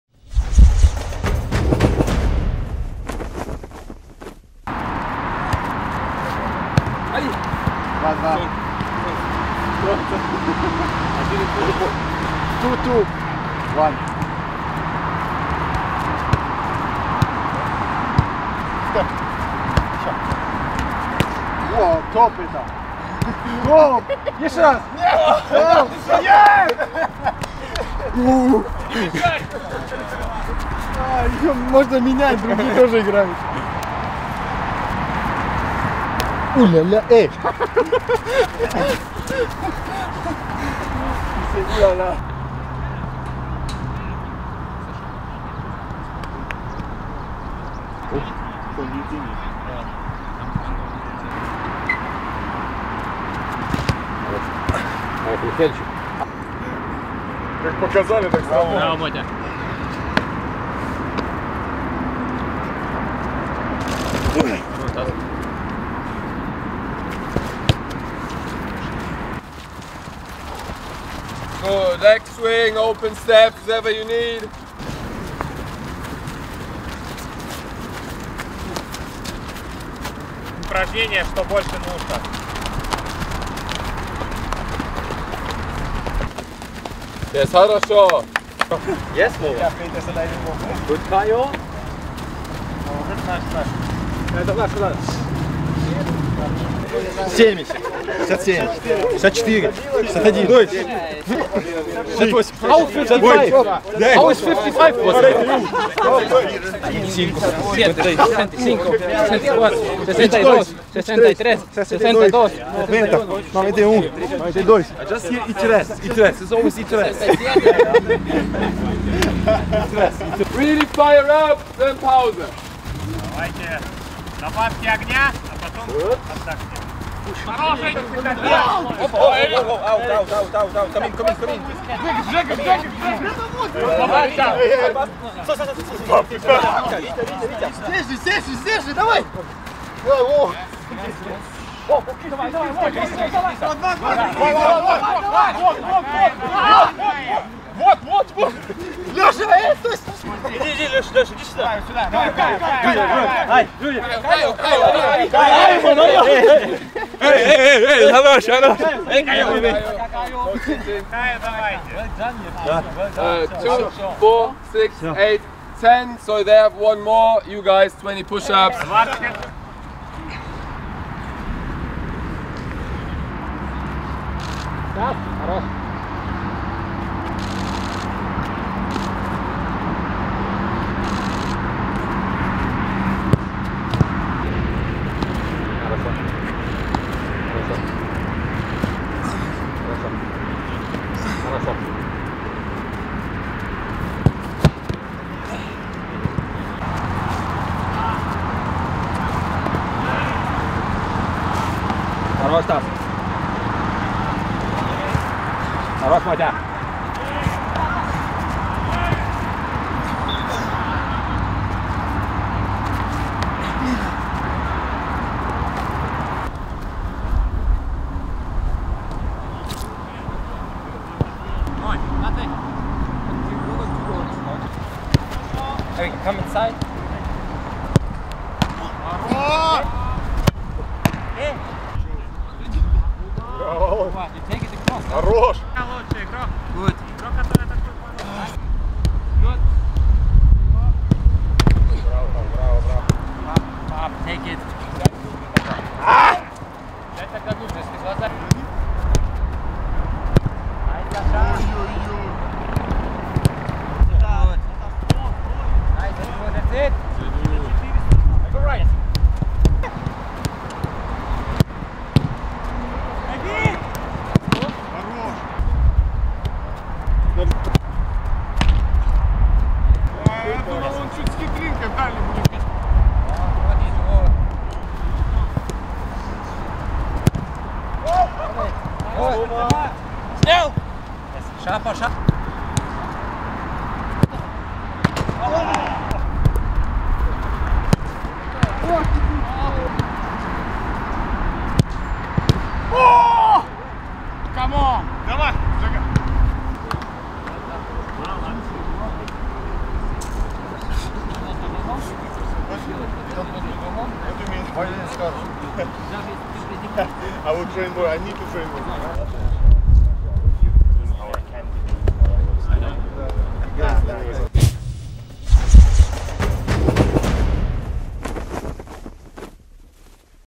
Занимательная арифметика и звонкие сейвы вратарского цеха
«Быки» провели последнюю тренировку на испанских сборах – впереди у «Краснодара» только контрольный матч с «Ростовом». Чёрно-зеленые поработали над контролем мяча, тактикой и ударами по воротам.